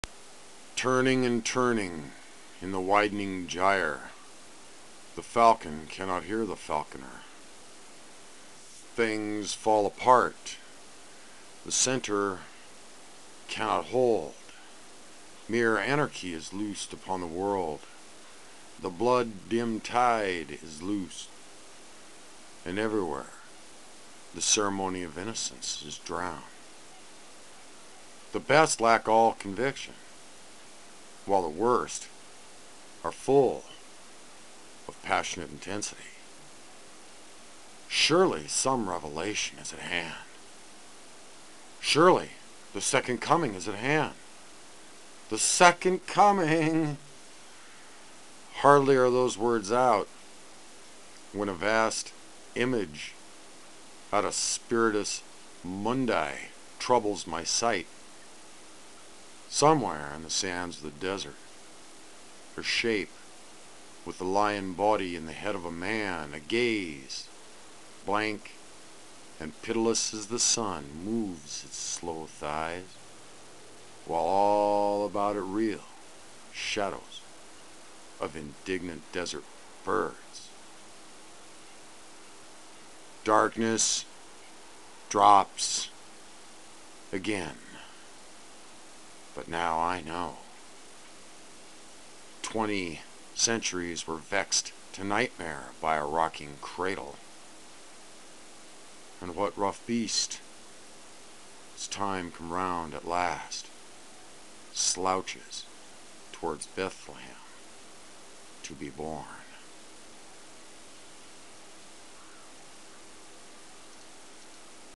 2nd Coming
Washington State poet Michael P Smith reads W. B. Yeats' "The Second Coming."